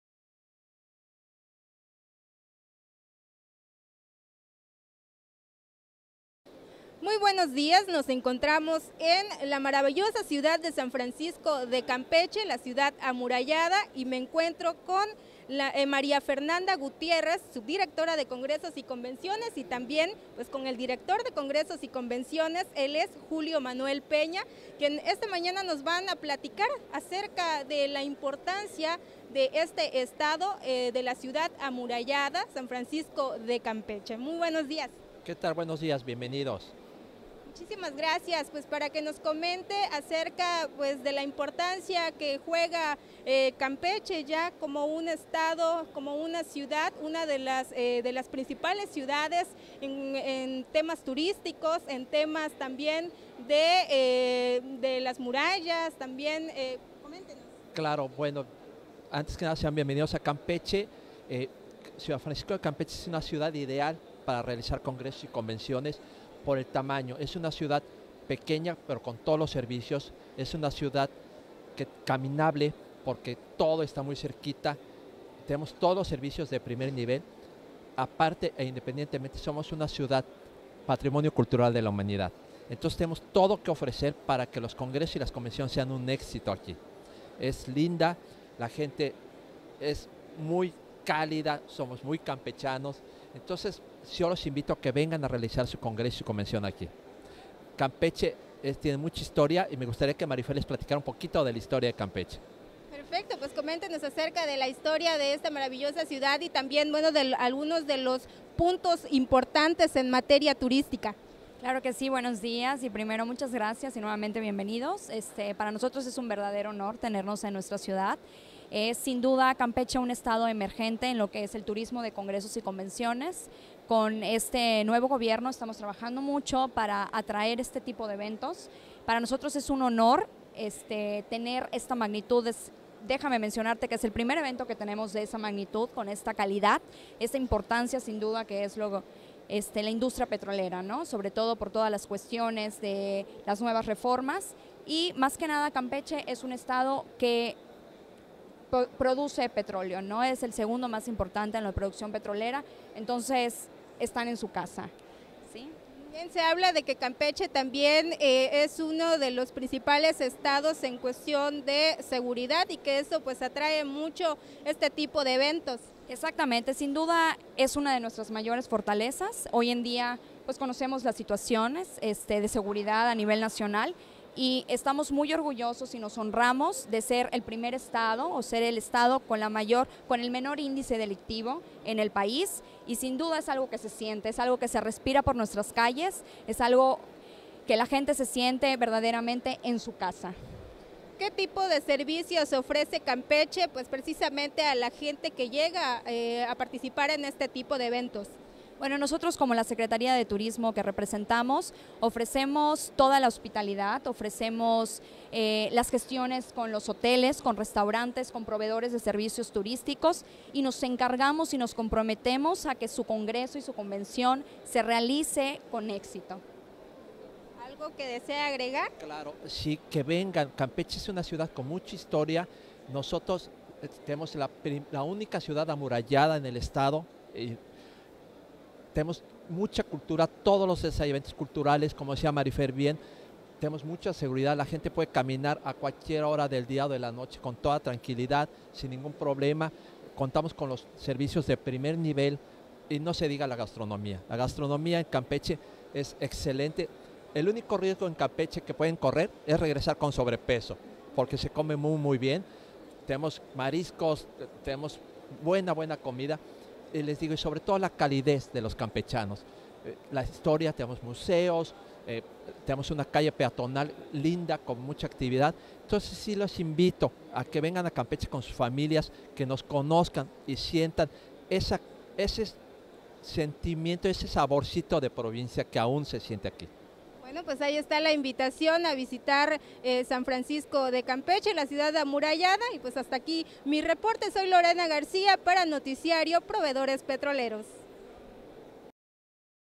TURISMO - CMDH2016
Entrevista a la empresa Congresos y Convenciones en el 2do Congreso de Medición de Flujo y Calidad de los Hidrocarburos 2016